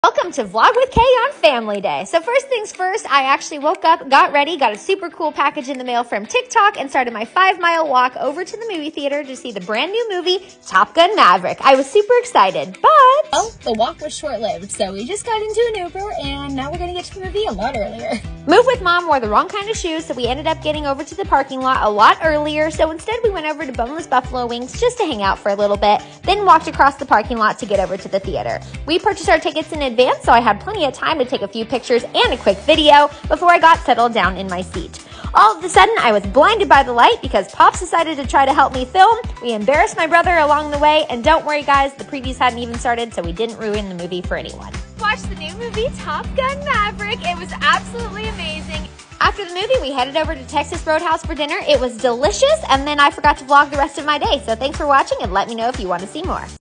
TopGun:Maverick with the family!
Here my family and I are spending the day together going to see the movie TopGun:Maverick, grabbing some food, and just having a great time.